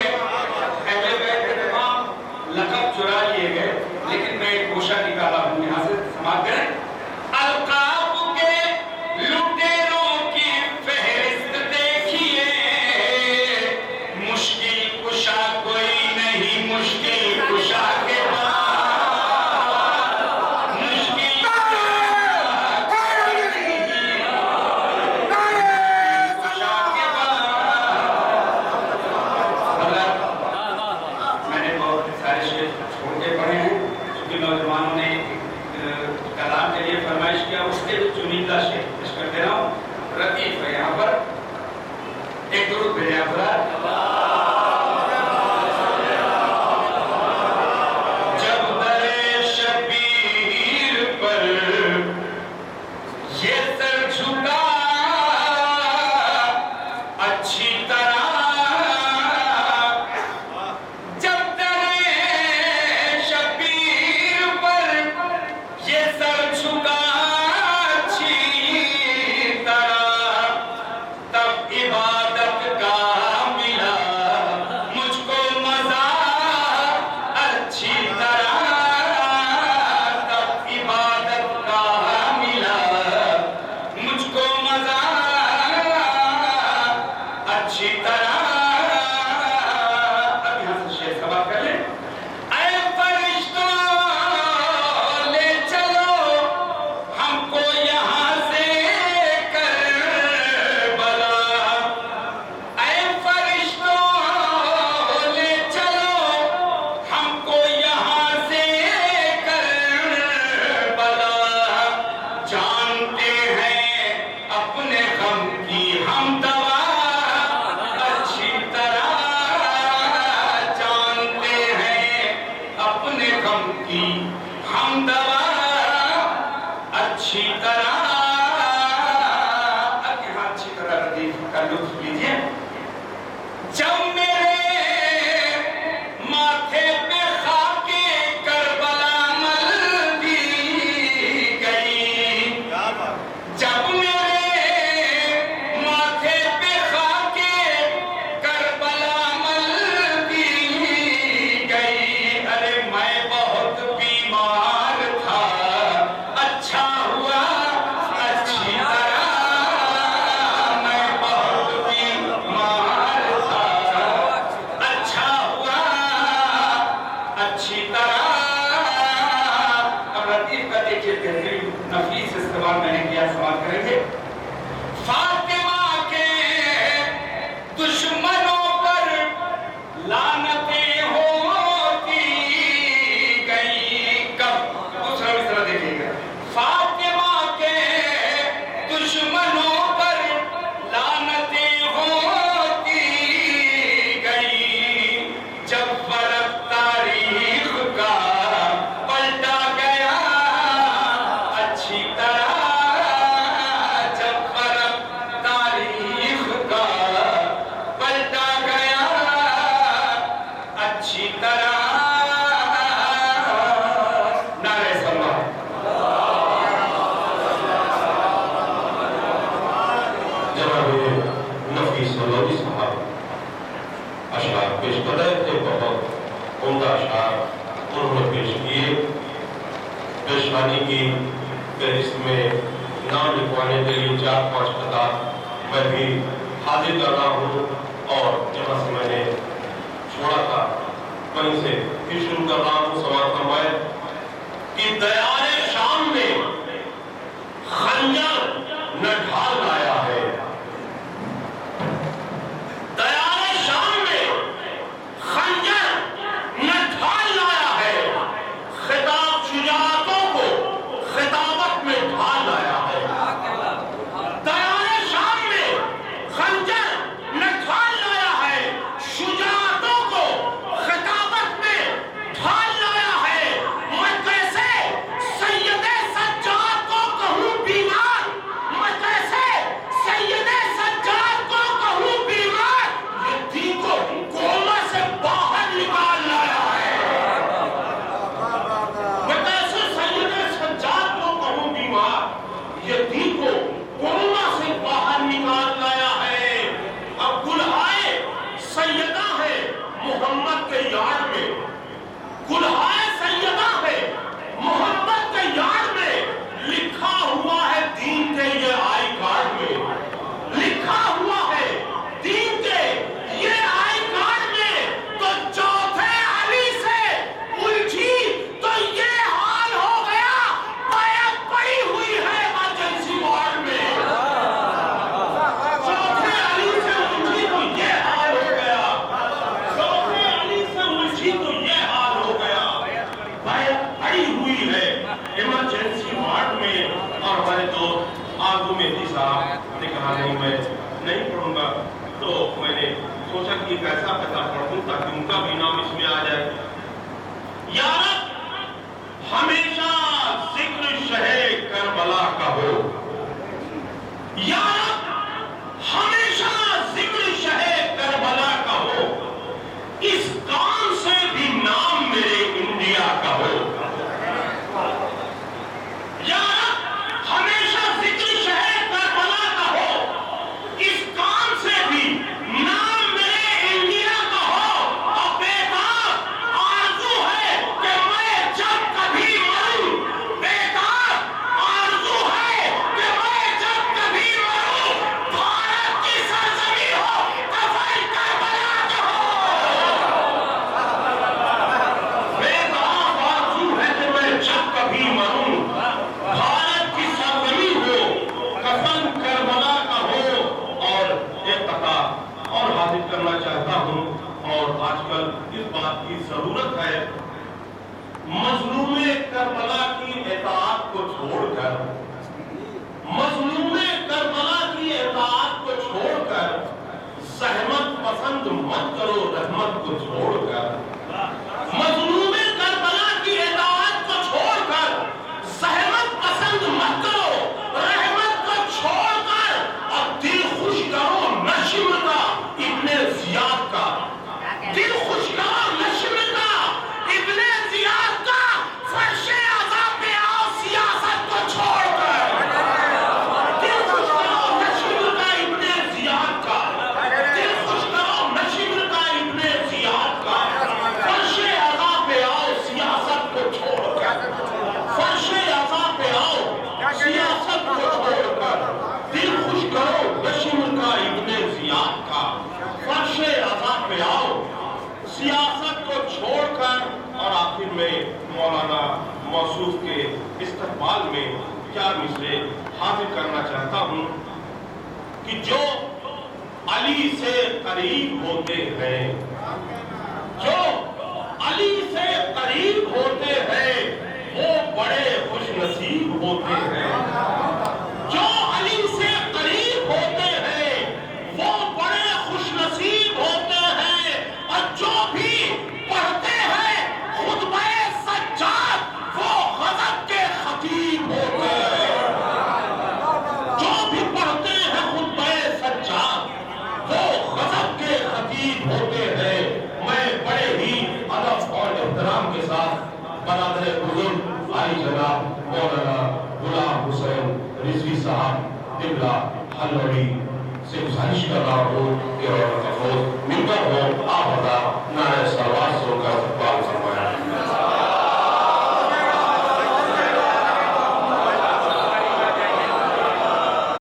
Peshkhaani - Nafees Hallauri at Babul Ilm, Okhla Vihar, Delhi 2024
A majlis was organised by Anjuman Haidari Hallaur, branch Delhi at Babul Ilm, Okhla Vihar, Jamia Nagar, New Delhi on 03-Aug-24.